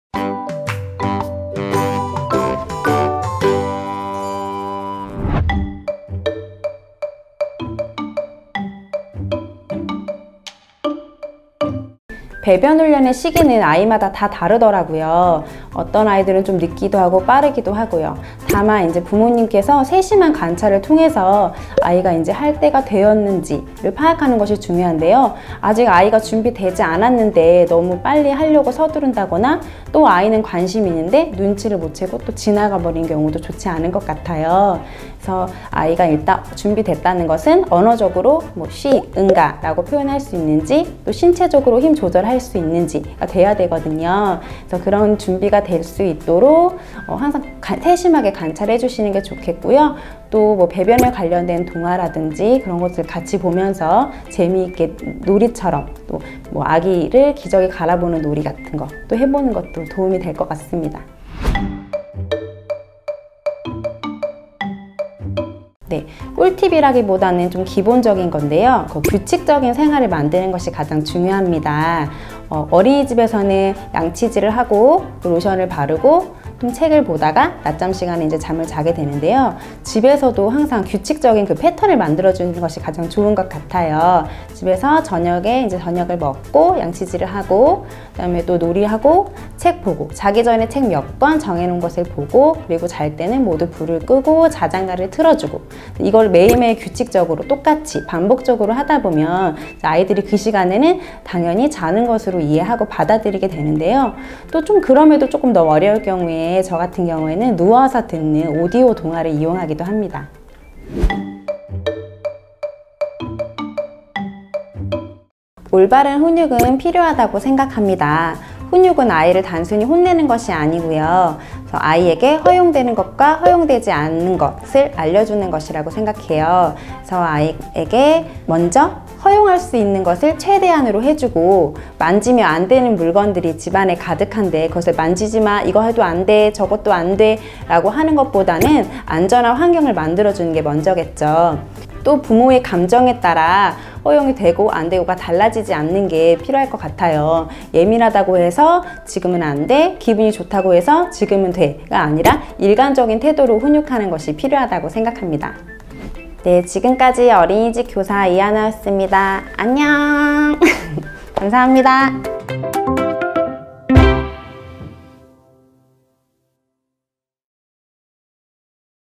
4~5세 생활습관 잡기! 어린이집 선생님에게 직접 물어봤습니다!
현직 어린이집 선생님께서 전해주는 4~5세 육아 꿀팁!!

배변훈련은 어떻게 하면 좋을까요?
어린이집 선생님이 말해주는 아이 재우는 꿀팁!
아이 훈육은 어떻게 하면 될까요?

육아를 하다보면 한 번쯤 궁금해지는 질문들을 모두 모아봤습니다~!

※ 현직 어린이집 선생님과의 Q&A 2편 보러 가기! :
※ 비룡소 북클럽 비버 구성 살펴보기!